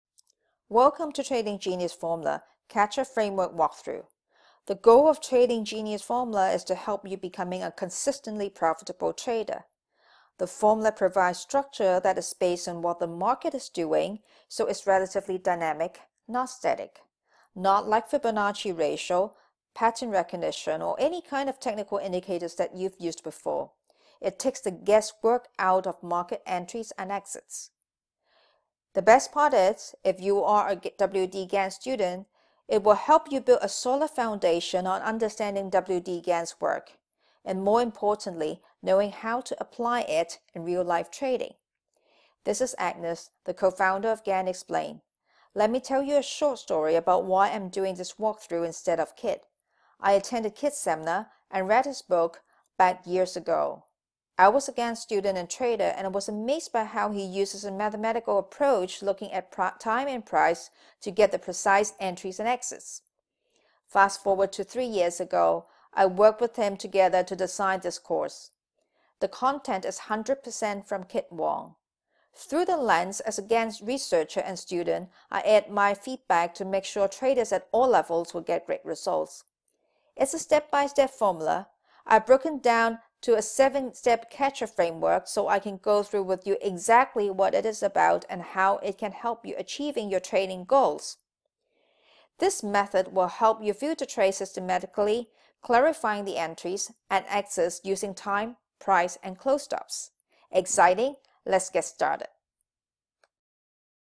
The W.D. Gann Trading System -The CATCHER Framework of Trading Genius Formula by Khit Wong
Inspired by Gann’s Law of Vibration, I’ve developed the Trading Genius Formula in which I use math to determine what the Market Has Done and is About To Do step by step, so you can implement it right away.

Being a Consistently Profitable Trader is so much easier when you know the 7-Step Trading Genius Formula CATCHER Framework -

C= Cyclic Law
A= A-Player Trader System
T =Time Turn
C= Close Stops (Dark Energy)
H= High Reward
E= Exit Highway
R= Repeated Moves

Trading Genius Formula is based on my 20 years of trading experience,  90% of Gann’s original manuscripts I collected throughout my research journey, and my Astro Vol One that accurately predicted the 6 Tops & Bottoms of the Gold Market between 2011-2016. (The Astro Vol. One is the book that got my name out at FX Street, the Tradersworld Magazine, and Technical Analysis of Stocks & Commodities in the USA.)

Uncovering Gann’s mathematical secrets alone have radically changed the way my students trade and successfully ended their frustrations of being inconsistent traders.

Exciting?  You can access to Module One of Trading Genius Formula 7 Days for just $1.